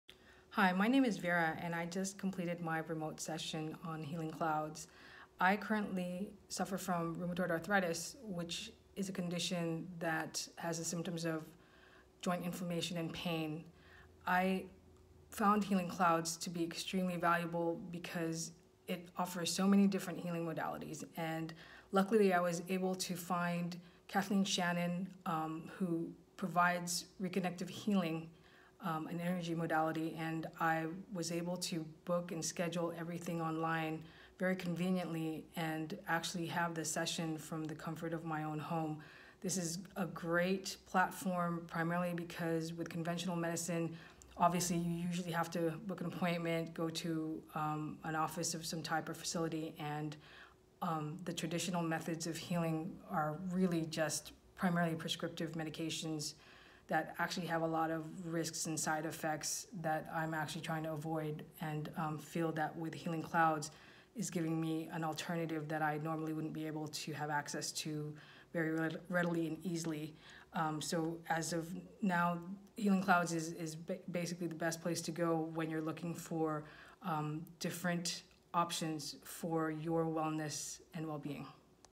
Client at Healing Clouds reviews Reverend Kathleen Shannon
After a session of Distance Healing online at healingclouds.com a client reviews practitioner Reverend Kathleen Shannon who facilitates Reconnective Healing. A link to her Healing Clouds profile is